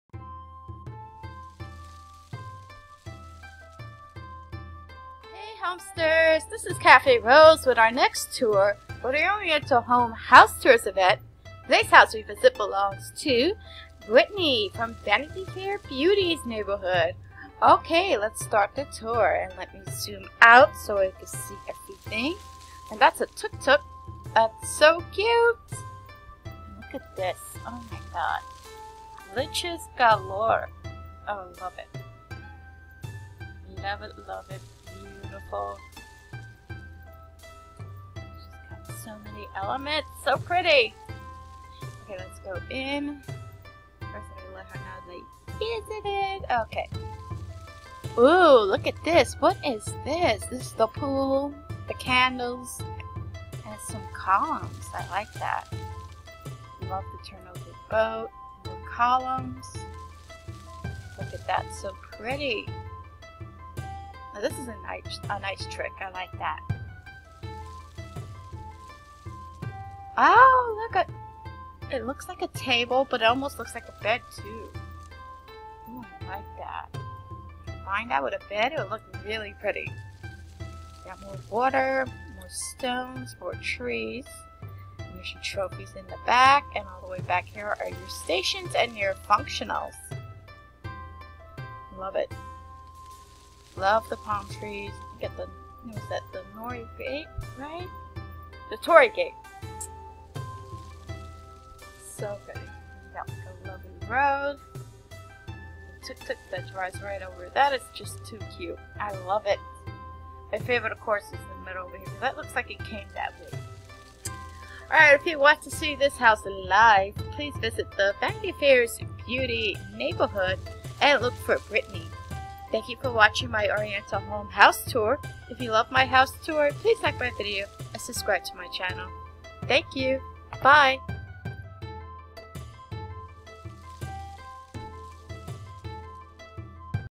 Oriental Home House Tours - Brittany
Hey, Homesters! Today's Oriental Home house tour is BRITTANY@VANITYFAIRBEAUTYS of the VANITYFAIRBEAUTYS neighborhood.